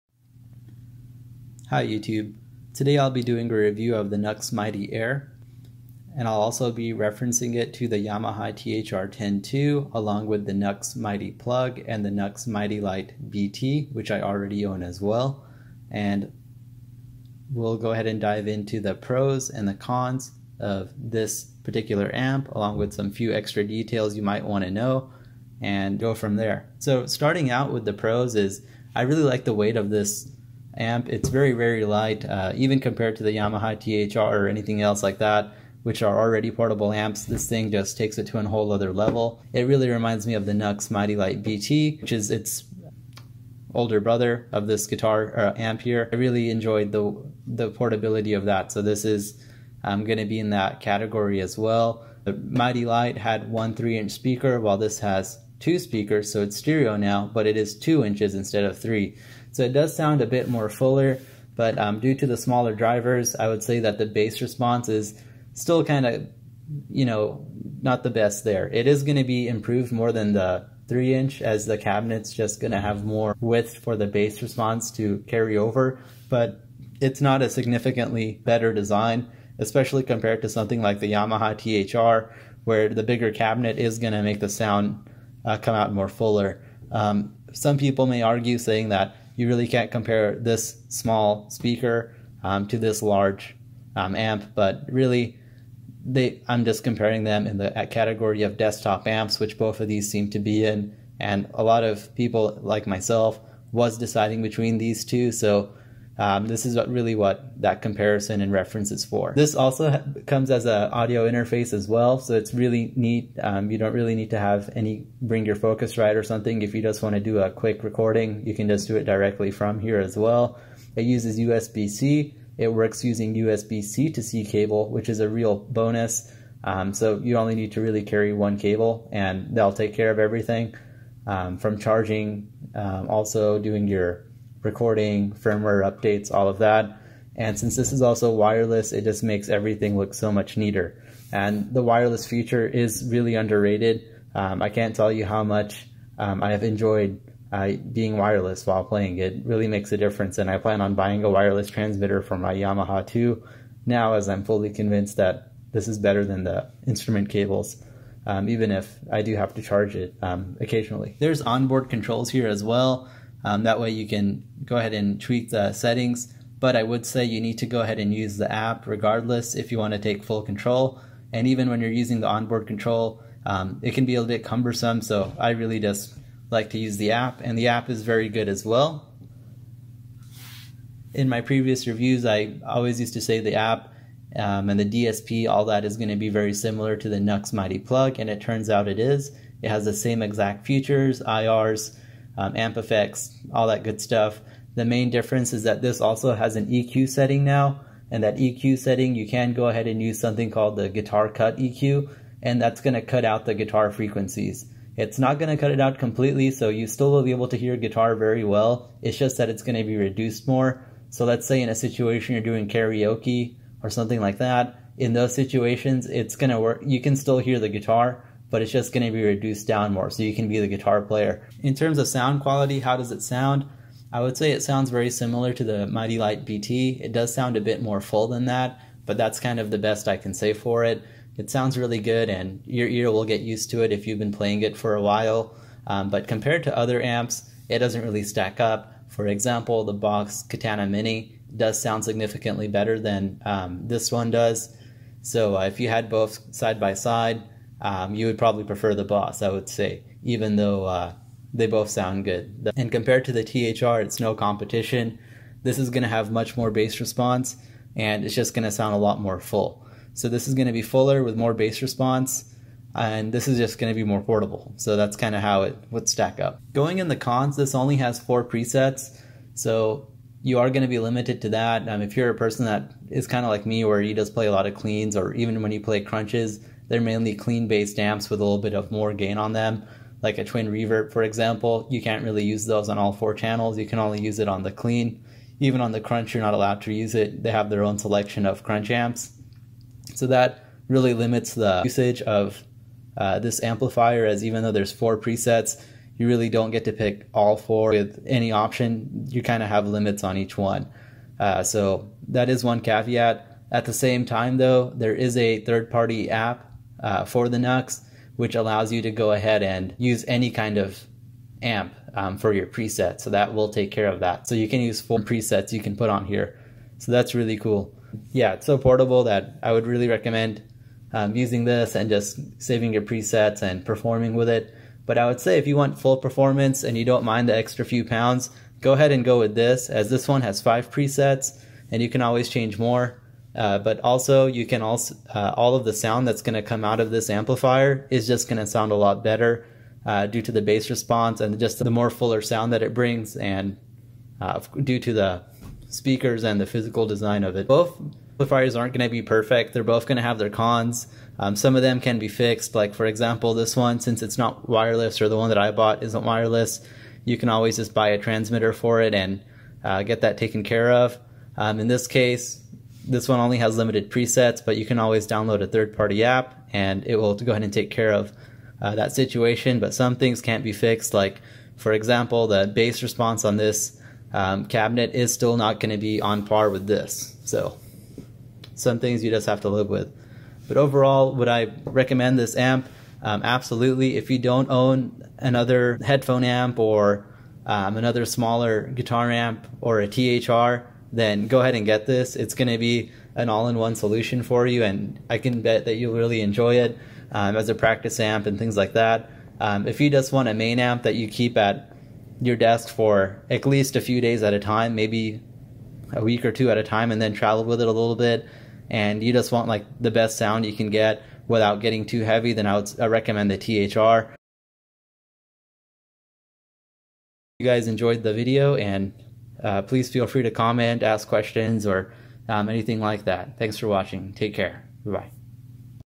Nux Mighty Air Review and Comparison
Nux Mighty Air Review, compared to the THR10ii and Mighty Plug